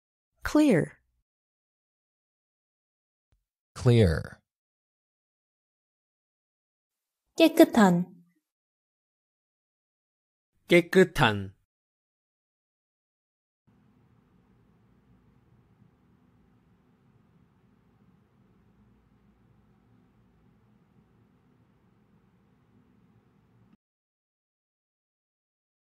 영어로 "깨끗한" 말하는 법
남자 목소리 :
Clear

여성 목소리 :
Clear

남성 음성 번역 :
깨끗한

여성 음성 번역 :
깨끗한